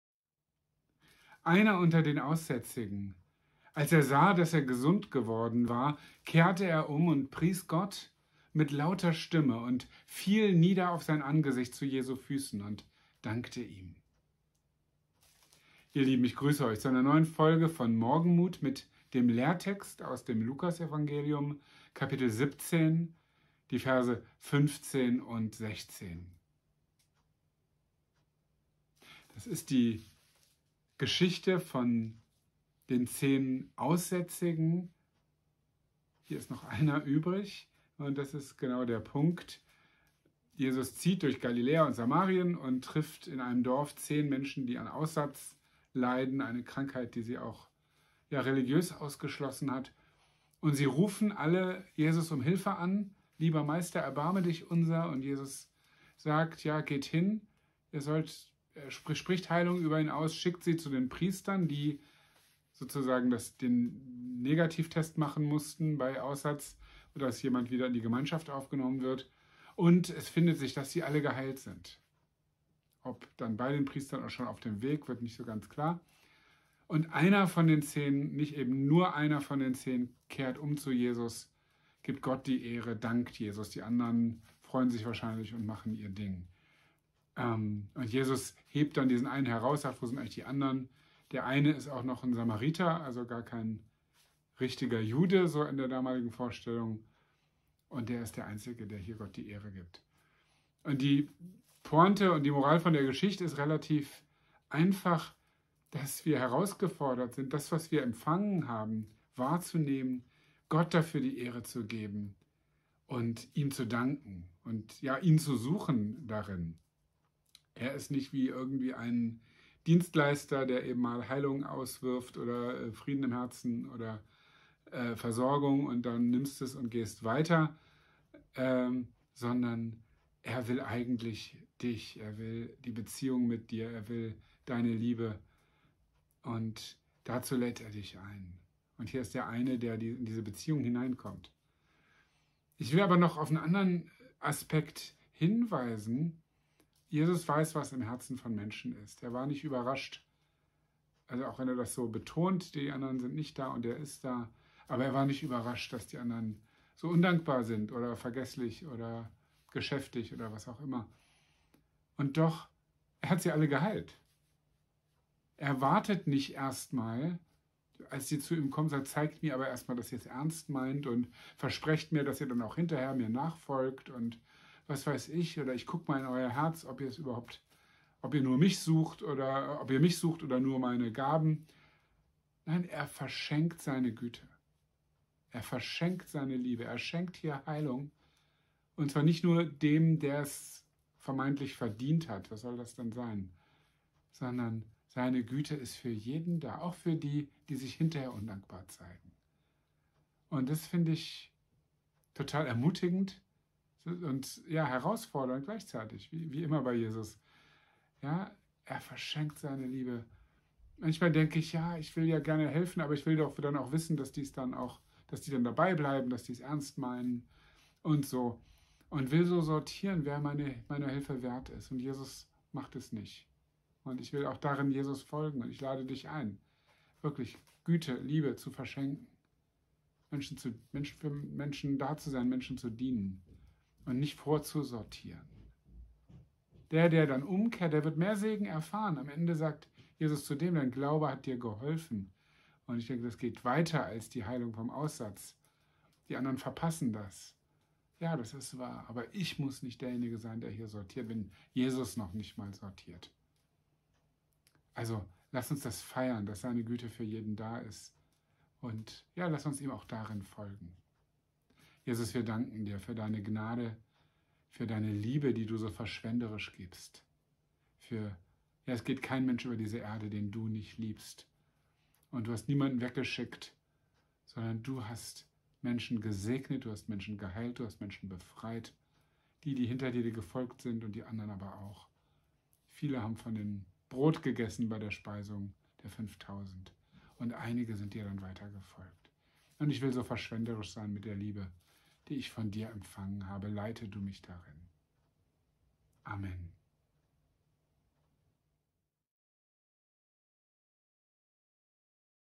"Einer unter den Aussätzigen kehrte um und pries Gott." | Morgenmut #408 | 27.07.2022
Eine tägliche Ermutigung in bewegten Zeiten.
"Einer unter den Aussätzigen, als er sah, dass er gesund geworden war, kehrte er um und pries Gott mit lauter Stimme und fiel nieder auf sein Angesicht zu Jesu Füßen und dankte ihm."
Lukas 17,15-16